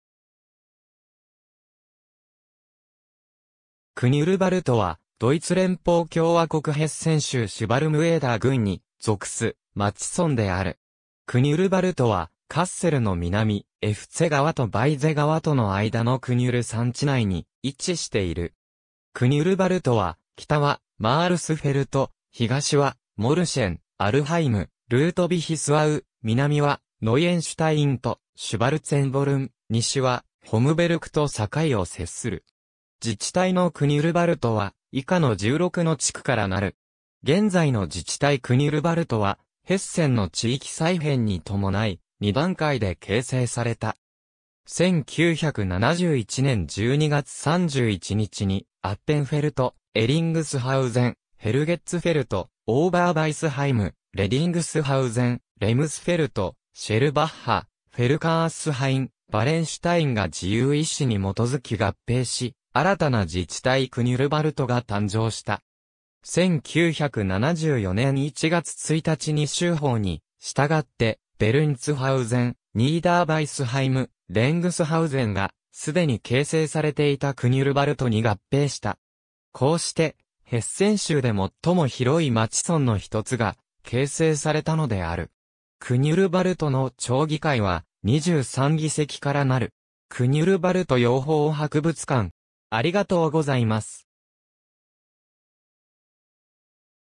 クニュルヴァルト
ドイツ・メルヘン街道
シュヴァルム＝エーダー郡
クニュルヴァルト (Knüllwald) は、ドイツ連邦共和国ヘッセン州シュヴァルム＝エーダー郡に属す町村（以下、本項では便宜上「町」と記述する）である。
クニュルヴァルトは、カッセルの南、エフツェ川とバイゼ川との間のクニュル山地内に位置している。
クニュルヴァルトは、北はマールスフェルト、東はモルシェン（ともにシュヴァルム＝エーダー郡）、アルハイム、ルートヴィヒスアウ、南はノイエンシュタイン（以上 3町村はヘルスフェルト＝ローテンブルク郡）とシュヴァルツェンボルン、西はホムベルク (エフツェ)（ともにシュヴァルム＝エーダー郡）と境を接する。
自治体のクニュルヴァルトは以下の 16 の地区からなる。
現在の自治体クニュルヴァルトは、ヘッセンの地域再編に伴い、２段階で形成された。
1971年12月31日にアッペンフェルト、エリングスハウゼン、ヘルゲッツフェルト、オーバーバイスハイム、レディングスハウゼン、レムスフェルト、シェルバッハ、フェルカースハイン、ヴァレンシュタインが自由意思に基づき合併し、新たな自治体クニュルヴァルトが誕生した。
1974年1月1日に州法に従って、ベルンツハウゼン、ニーダーバイスハイム、レングスハウゼンがすでに形成されていたクニュルヴァルトに合併した。
こうして、ヘッセン州で最も広い町村の一つが形成されたのである。
クニュルヴァルトの町議会は、23議席からなる。
クニュルヴァルト養蜂博物館